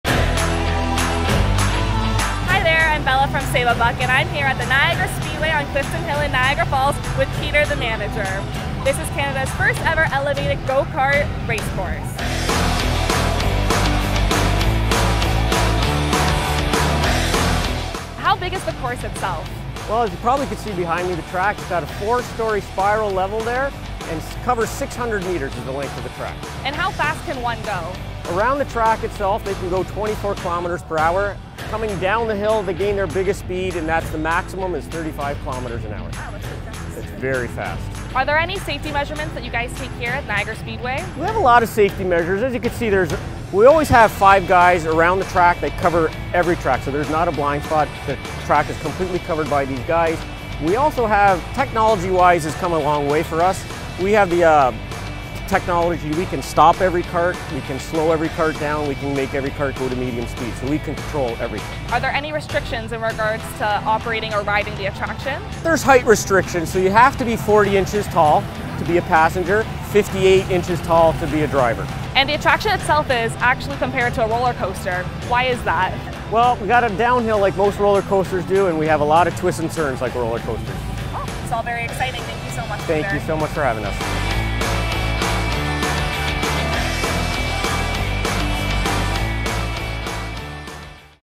Niagara Speedway - Canada's First Elevated Go - Kart Course
It’s tall, it’s fast, it roars with excitement! Canada’s first ever elevated go-kart race course is NOW OPEN!

The Speedway is a world-class attraction that combines the adrenaline inducing thrill of kart racing with the remarkable experience of a roller coaster ride.